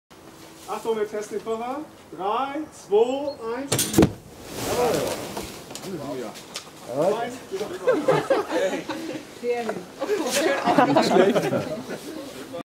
Kofferbomber im Sonderlehrgang Pyrotechnik 2013
Übungsaufgabe im Sonderlehrgang Pyrotechnik 2013 der Pyrotechnikerschule